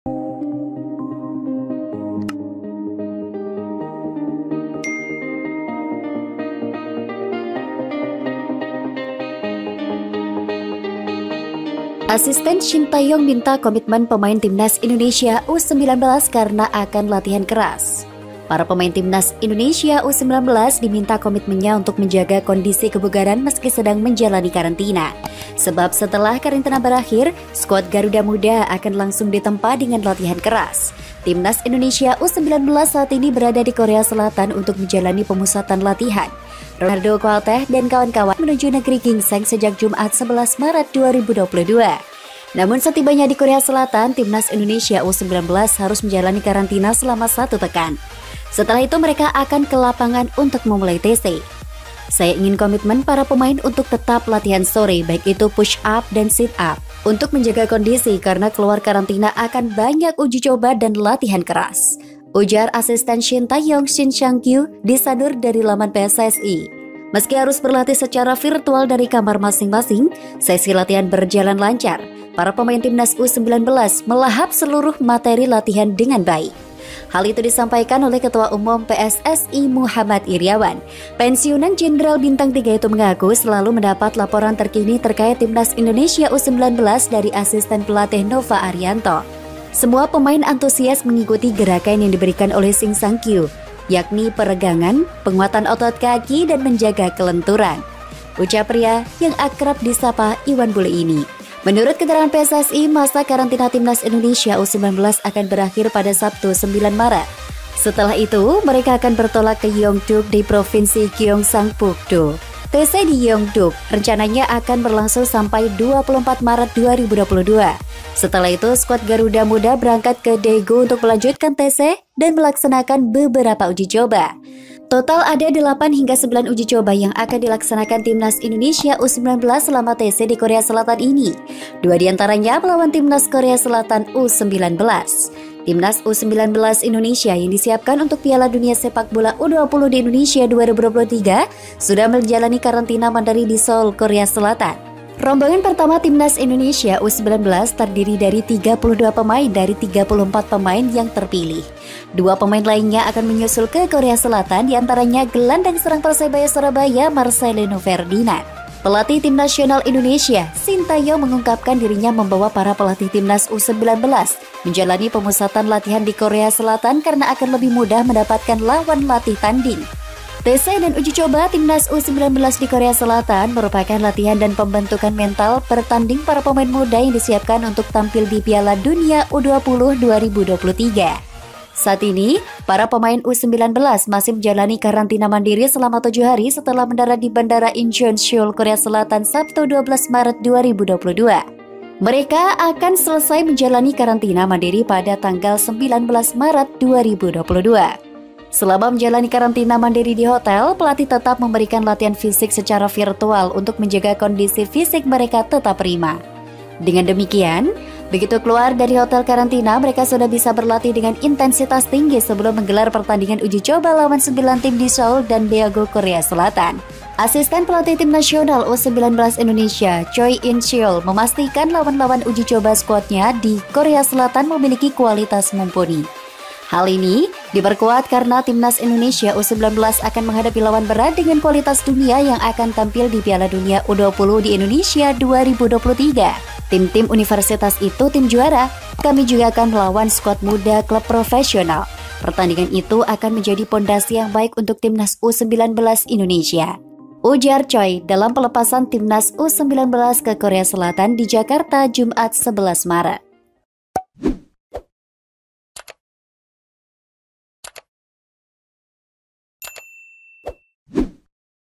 PERINGATAN KERAS 🔴ASISTEN PELATIH STY INGATKAN PEMAIN TIMNAS U19 KARENA LATIHAN AKAN SANGAT KERAS🔥
deskripsi:
Para pemain Timnas Indonesia U-19 diminta komitmennya untuk menjaga kondisi kebugaran meski sedang menjalani karantina. Sebab, setelah karantina berakhir, skuad Garuda Muda akan langsung ditempa dengan latihan keras.

Timnas Indonesia U-19 saat ini berada di Korea Selatan untuk menjalani pemusatan latihan (TC). Ronaldo Kwateh dan kawan-kawan bertolak menuju Negeri Ginseng sejak Jumat (11/3/2022).

Namun setibanya di Korea Selatan, Timnas Indonesia U-19 harus menjalani karantina selama satu pekan. Setelah itu, mereka akan ke lapangan untuk memulai TC.

"Saya ingin komitmen para pemain untuk tetap latihan sore, baik itu push-up dan sit-up. Untuk menjaga kondisi karena keluar karantina akan banyak uji coba dan latihan keras", ujar asisten Shin Tae-yong, Shin Sang-gyu, disadur dari laman PSSI, Rabu (16/3).

Meski harus berlatih secara virtual dari kamar masing-masing, sesi latihan berjalan lancar. Para pemain Timnas Indonesia U-19 melahap seluruh materi latihan dengan baik.

Hal itu disampaikan oleh Ketua Umum (Ketum) PSSI, Mochamad Iriawan. Pensiunan jenderal bintang tiga itu mengaku selalu mendapat laporan terkini terkait Timnas Indonesia U-19 dari asisten pelatih, Nova Arianto.

"Semua pemain antusias mengikuti gerakan yang diberikan oleh Shin Sang-gyu, yakni peregangan, penguatan otot kaki, dan menjaga kelenturan,” ucap pria yang akrab disapa Iwan Bule ini.